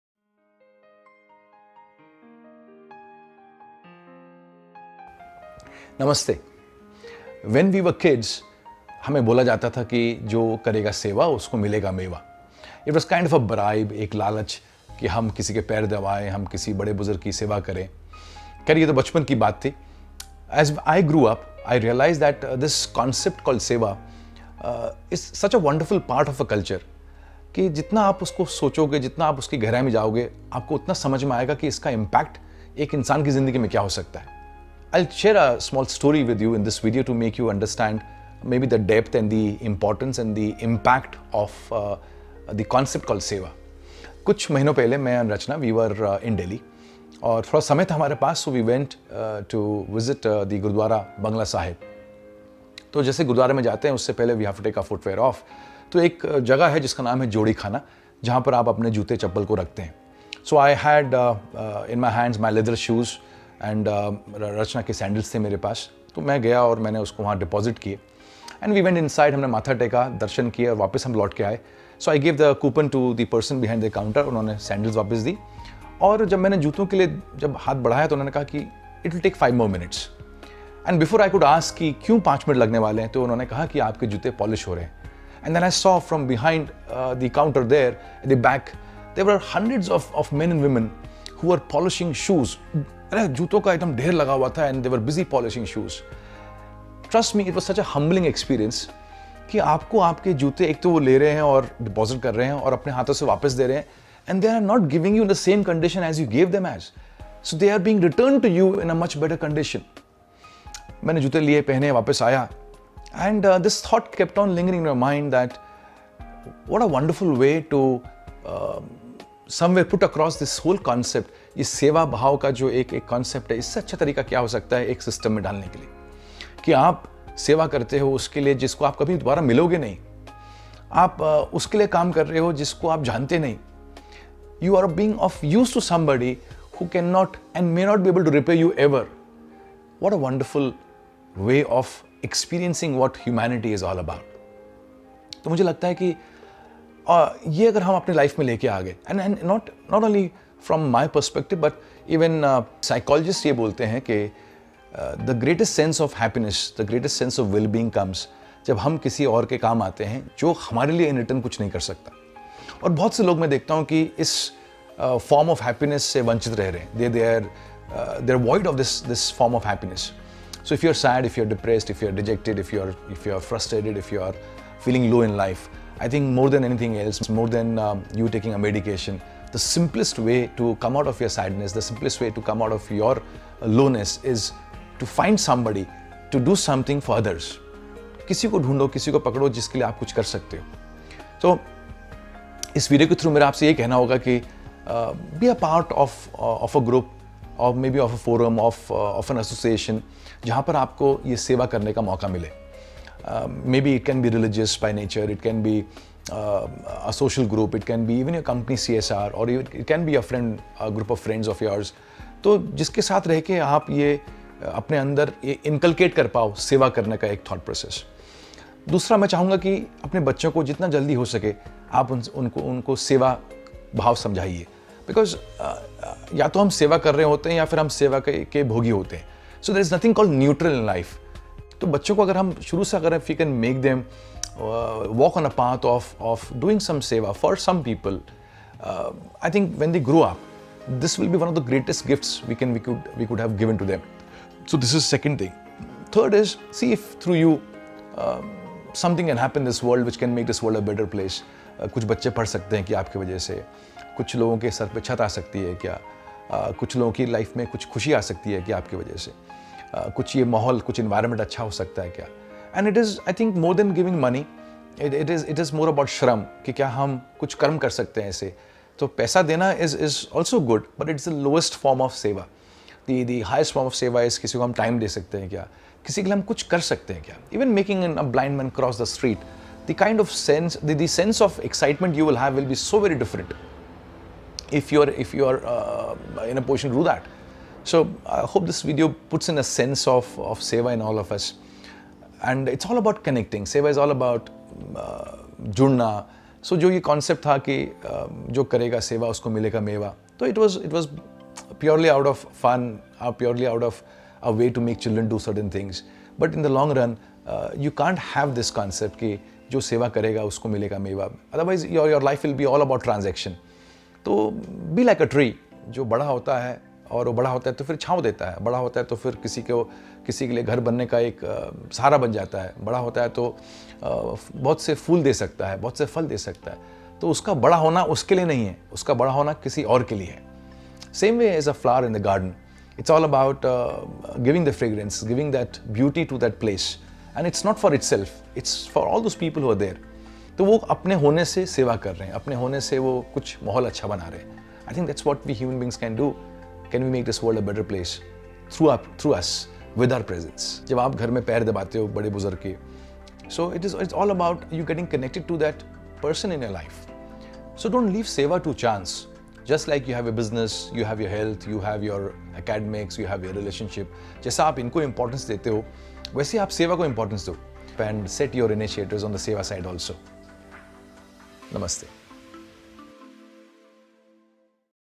Jo Karega Seva..| Manish Gupta.
If you look around you’ll see that everything present in the world ( except mankind) is in some way serving the world. So the trees don’t have fruits for themselves, the flowers don’t bloom for themselves, the rain doesn’t fall for itself. everything in the universe is in some way serving somebody else...

Can we pick up this attribute from nature and be of service to the world? In our culture, we call it Seva & it has been a very integral part of our lives. It’s a simple act of being of use to someone else.

And it’s been found out that the greatest sense of wellbeing comes when we are of use to someone who cannot repay us back. In fact, Seva is the fastest way to come out of sadness, depression, and loneliness.

Can we see our work as a way to serve the world?
Can we find ways to serve the world?
Can we inculcate it as a part of our lives?